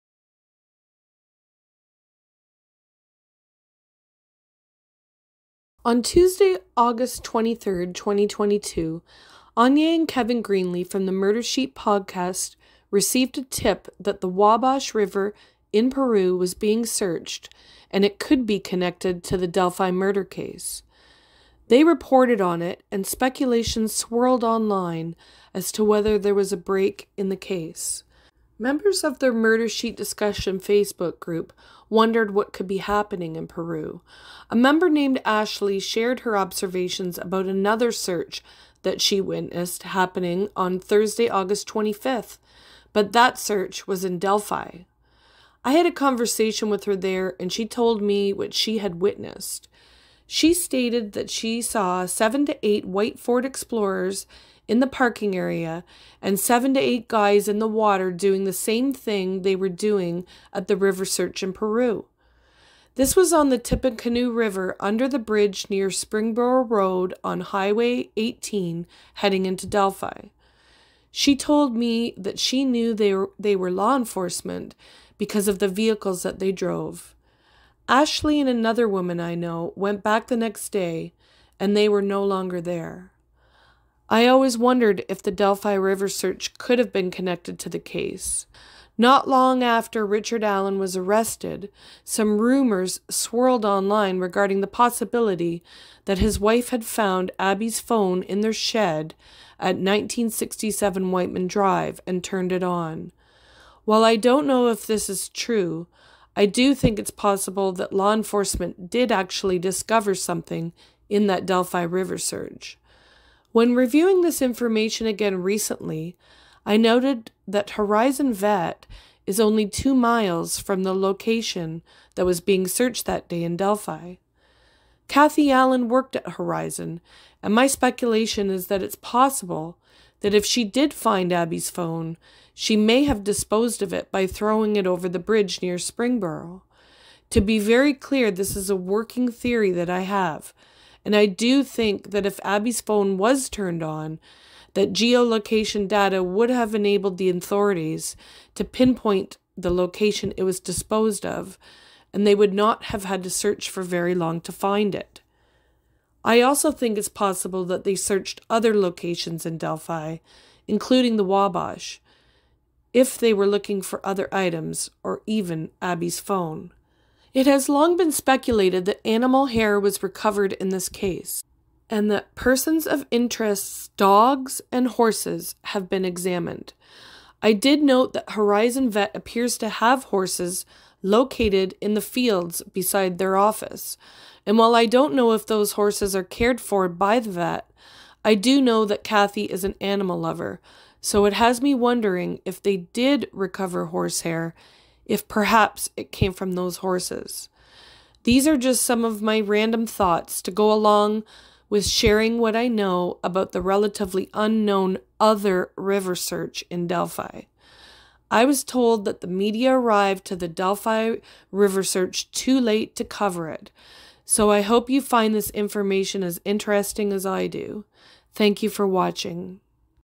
The Delphi River Search
Law enforcement were seen searching the Tippecanoe River under the Springboro Bridge on Highway #18 heading into Delphi, Indiana on Thursday, August 25, 2022. Could Abigail Williams phone have been tossed there?